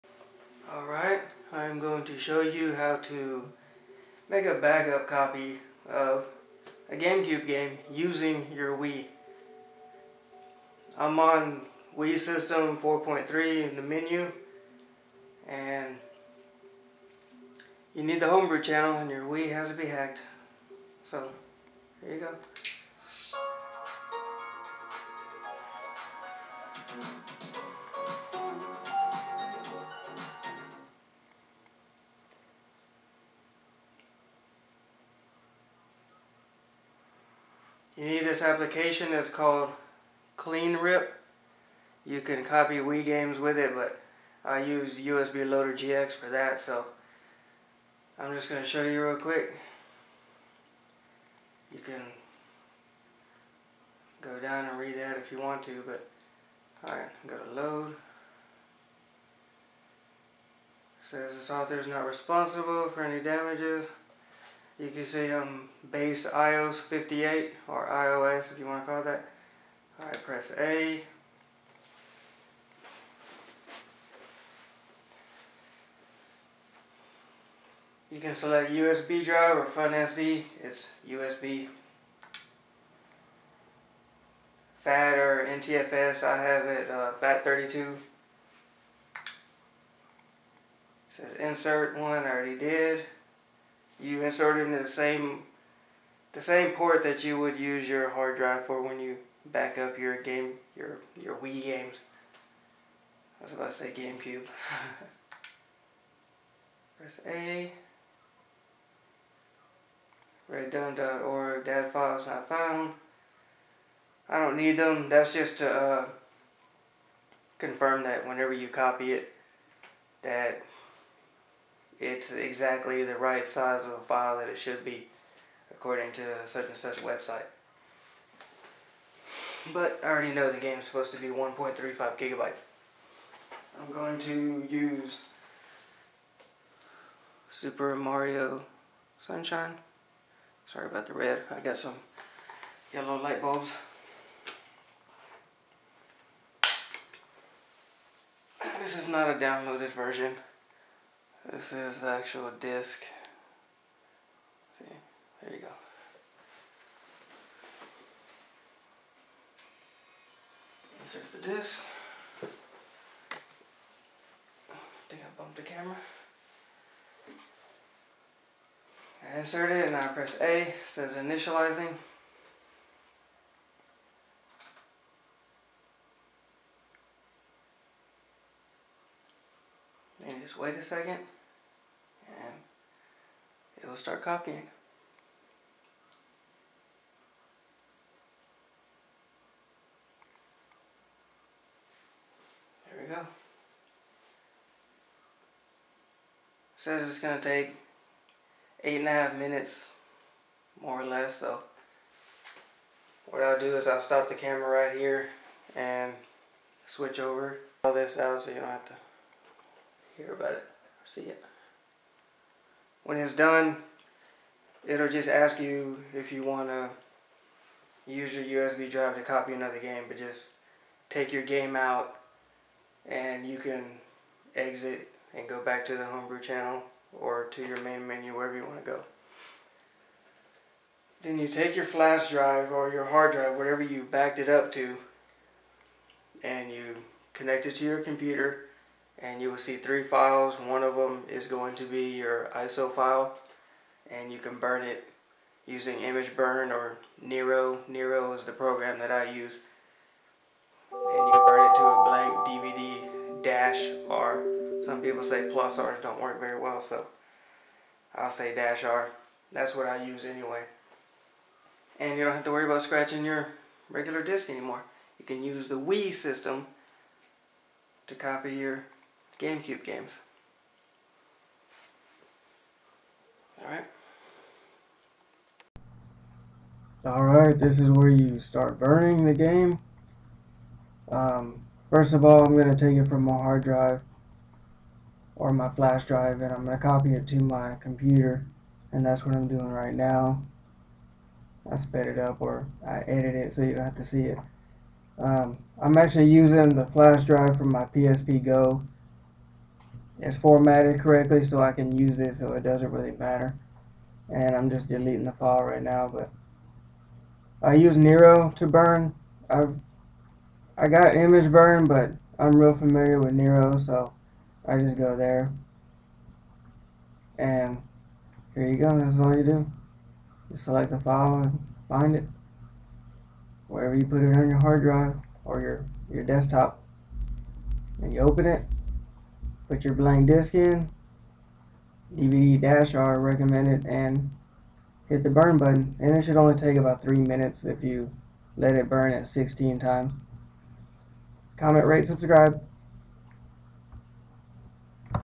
[HOW TO]: Backup GAME CUBE Games Using the Wii/ CleanRip-v.2.0.0 PLUS LINK!
I use v.1.0.4 in the video. Follow down below and you can get v2.0.0 which is the newest.

You MUST have a Wii.
It MUST have the HomeBrew Channel Installed.
You NEED either an SD Card, Flash Drive, or HDD with at LEAST 2GB FREE.
It MUST be formatted to FAT32 or NTFS. WBFS will NOT work.
*NOTE- This does NOT allow Game Cube Loading from USB.
Only BACKUP Games YOU OWN.
You NEED base IOS58 installed.
To play the Backup, your Wii MUST be modded to read DVD-R Discs.
It ALSO MUST be patched for Game Cube DVD-R Backup.

When finished, it will place the "Game Title".ISO on your USB drive or SD card if you chose that option.
Copy that to your computer, and use Nero or IMGBurn to burn the disc to a DVD-R. NOT +R. You can use any program that recognizes .ISO files to burn it also.

BTW, speed of burning HAS NO EFFECT on the quality of the disc. It depends what your computer and or DVD Drive can handle. No matter what anyone says, it does NOT matter. Do the research if you wish.

If you installed Darkcorps on your Wii, then it will read the Backup right through the Disc Channel.

Thanks to DutsonEntertainment for his hacking tutorials.

Sorry if my text here sounds like I'm angry. Not my intention.

Whatsnew in 2.0.0:
- GameCube version integrated into one codebase
- GX GUI
- Fix disc dumping on Wii U (Wii discs only of course)
- Fix ETA calculation on non GC discs
- Fix fatUnmount
- Stop larger than 4GB chunks on FAT FS
- Fix potential issue when read error occurs